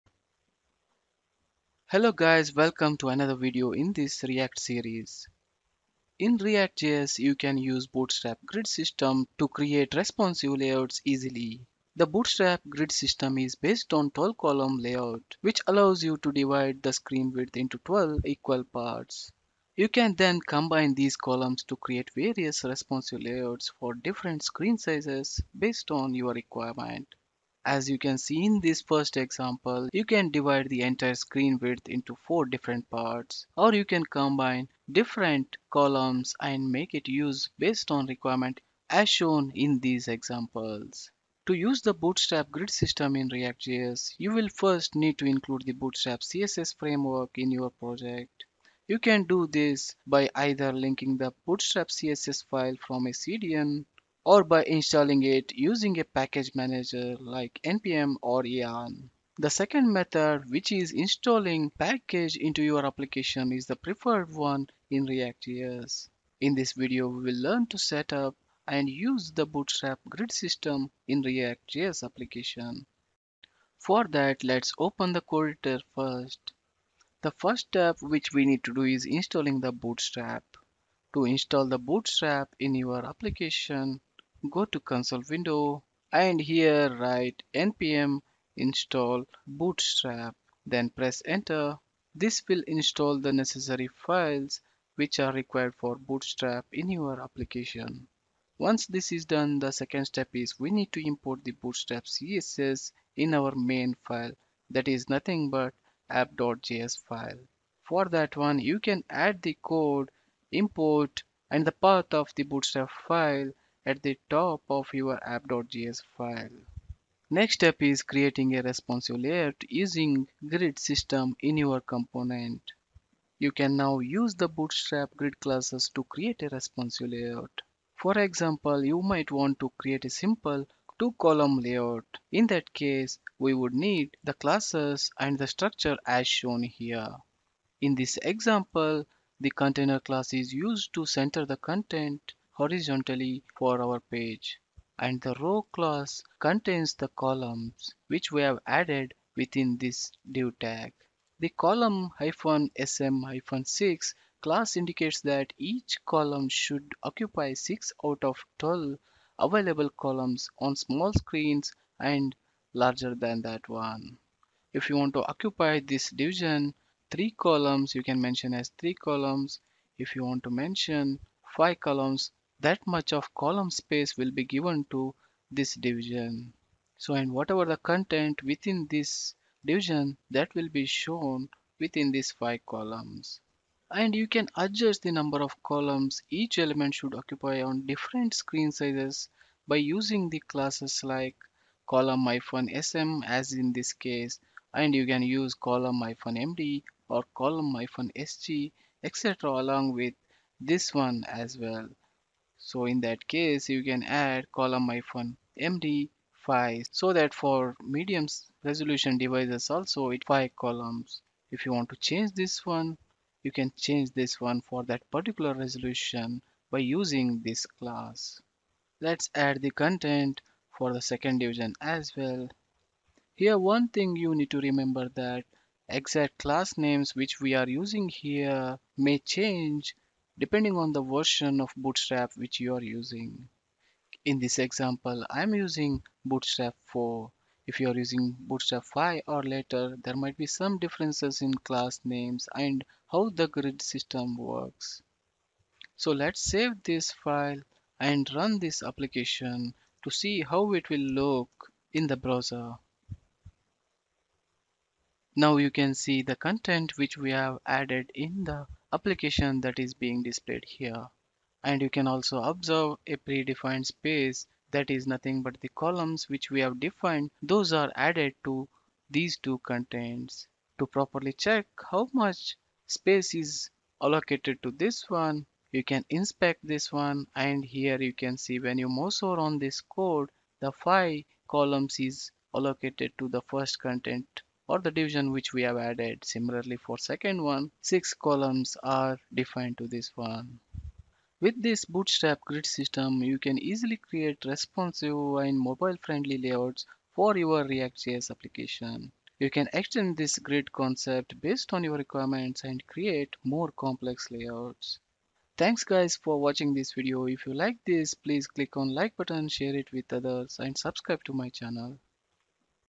ReactJS + Bootstrap: Bootstrap Grid system in ReactJS
In this video you will learn the basics of Bootstrap Grid system in ReactJS Application along with an example.